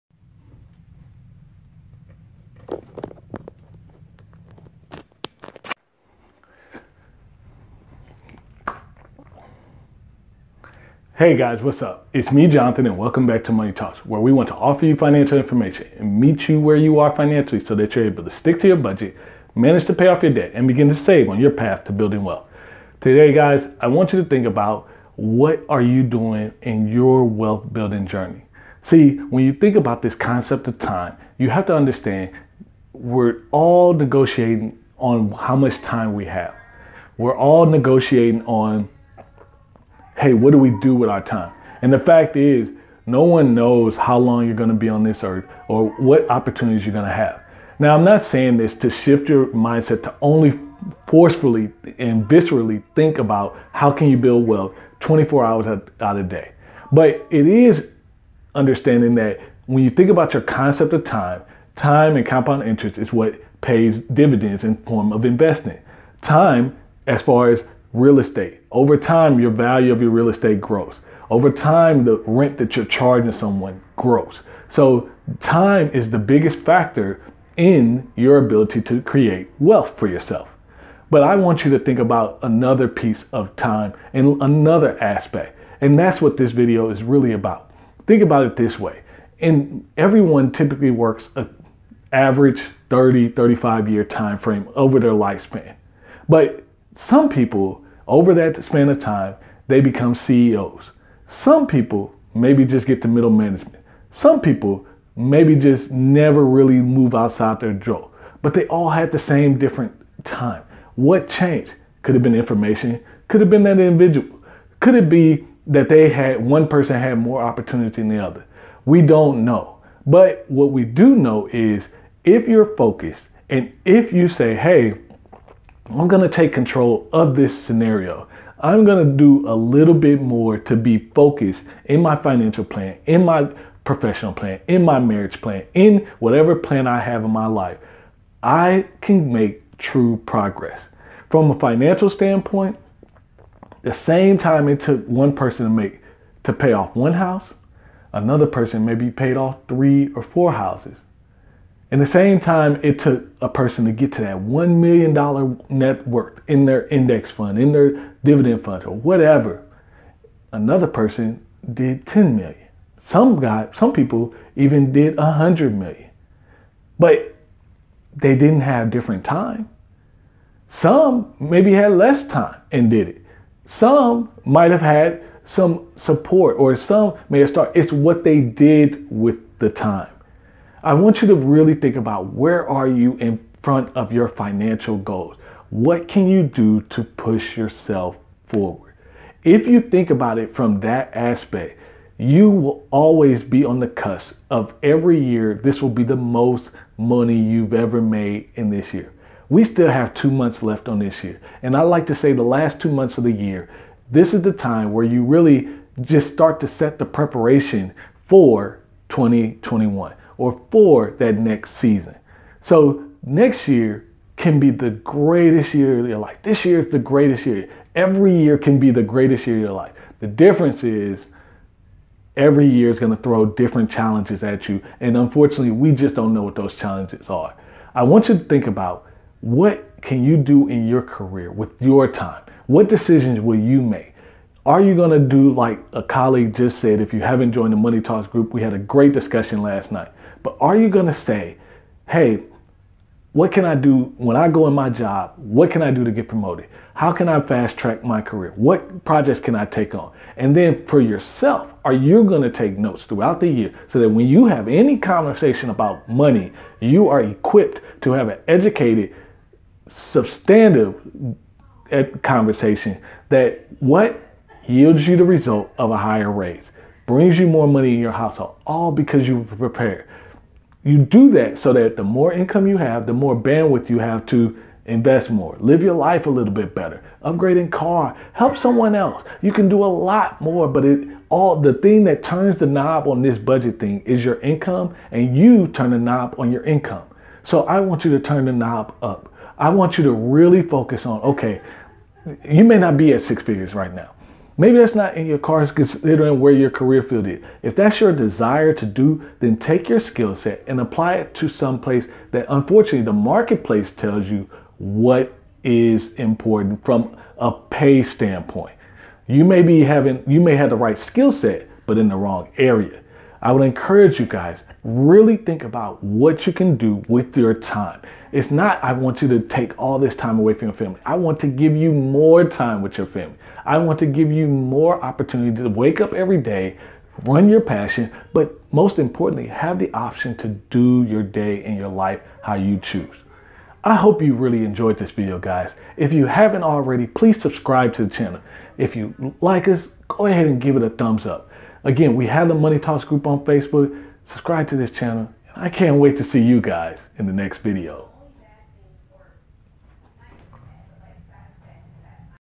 How can Time work for you?
How can you make time work for you to improve your circumstance.

Do you have a career progression plan and are you seeking opportunities to grow your income.
Do you understand your investments.
Are you maximizing the time you have to grow your wealth.